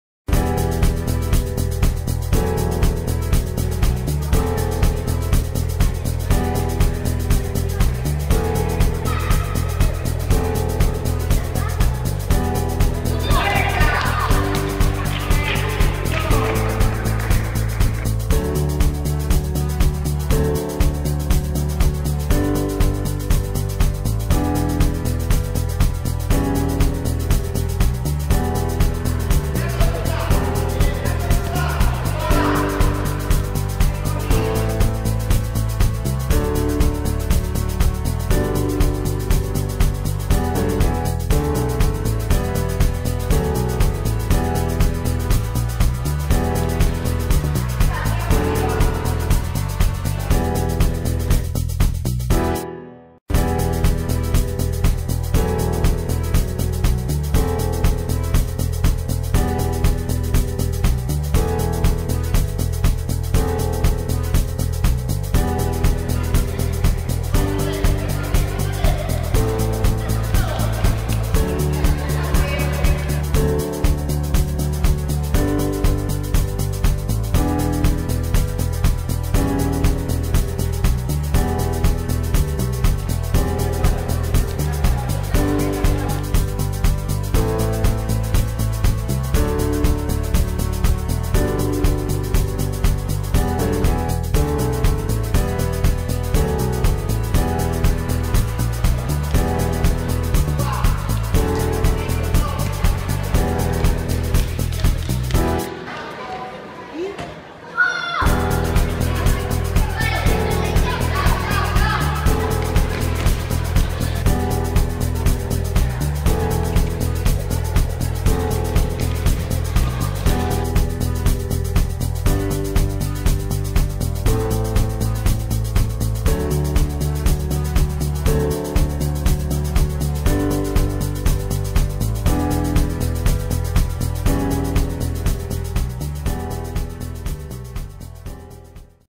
REGATES Y GAMBETAS. ESCUELA DE FUTBOL. SOCIEDAD ITALIANA
Jugadas, gambetas, regates y asistencias de Santiago en un dia mas de entrenamiento en la escuela de futbol de Sociedad Italiana.